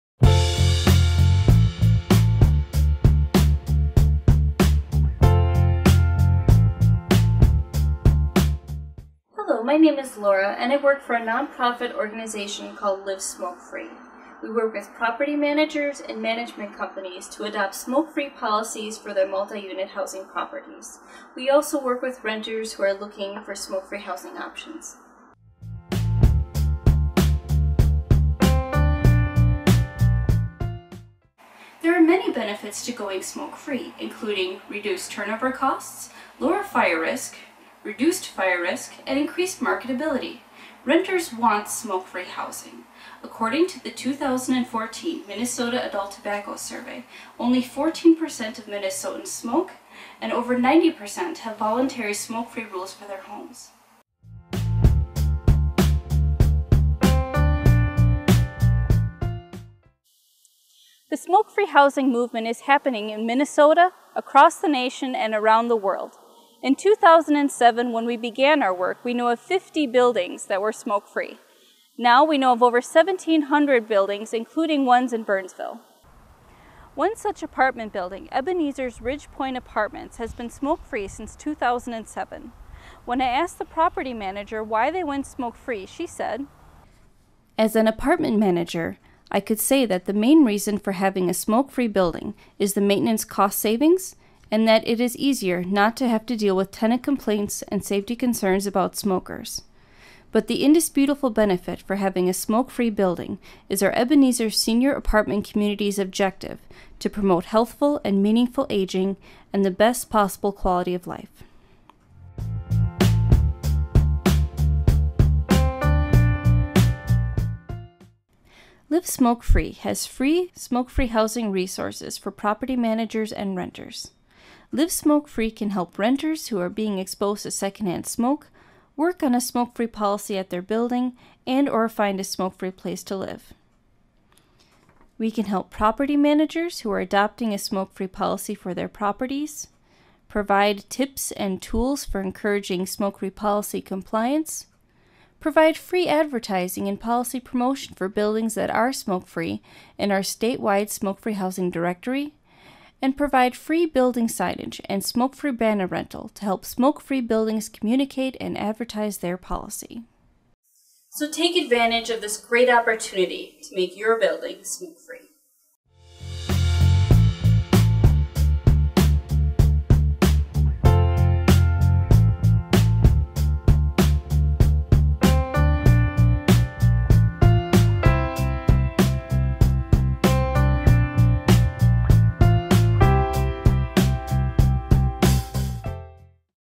Smoke-Free Housing is Heating Up in Burnsville
Live Smoke Free has free resources for Dakota County property managers and renters. See how things are heating up in Burnsville (MN) and find out what we can offer!